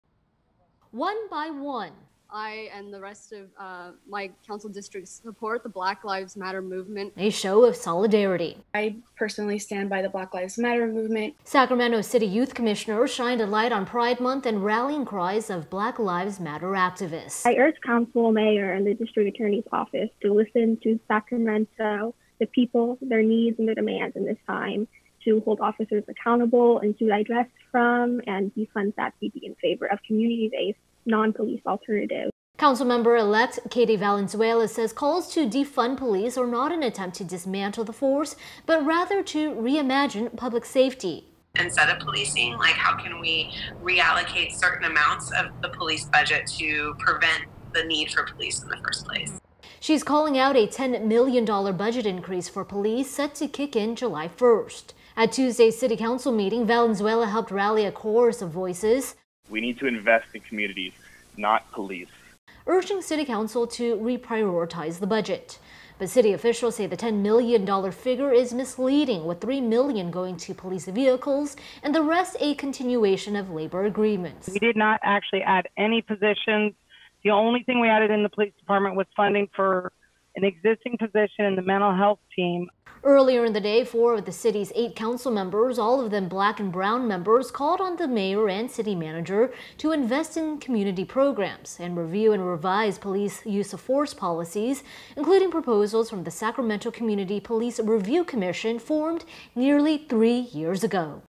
Sacramento joint youth commission meeting furthers calls to 'defund police'
In a special joint city council and youth commission meeting, the call to "defund police" and invest in community programs echoed.